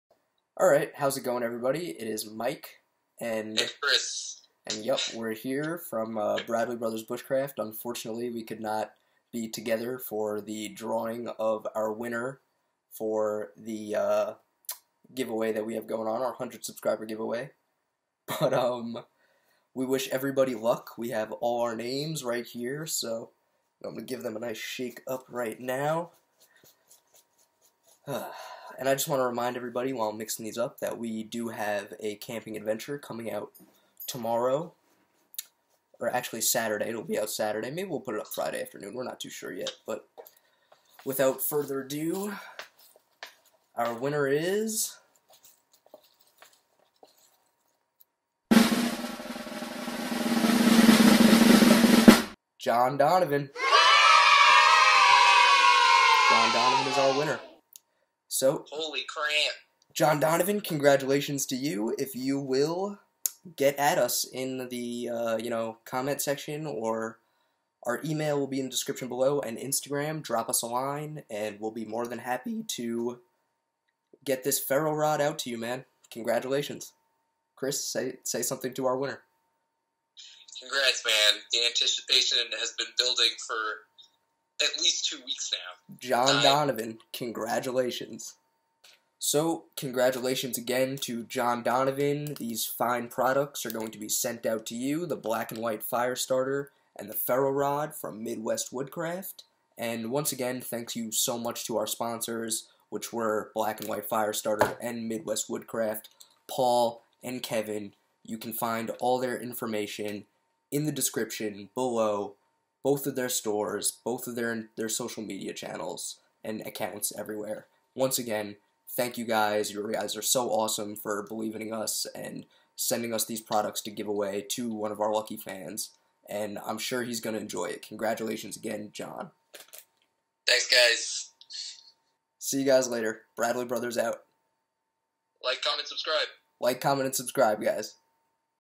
100 Subscriber Giveaway Winner Announcement!!!
A huge THANK YOU!!! to EVERYONE who entered this contest and who supports us!

Congratulations to our lucky winner!!!

Midwest Woodcraft

Black and White Fire Starter

On the Web:
MrOutdoorsy

For more Bradley Brothers follow...